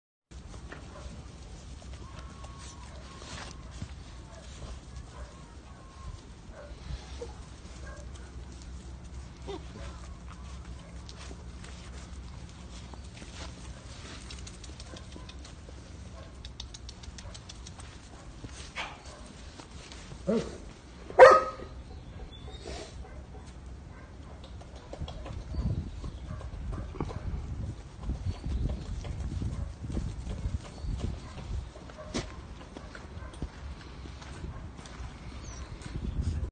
【出会い】ワイマラナーの仔犬とドーベルマン
お散歩中にドーベルマンに遭遇！
ワイマラナーの仔犬達です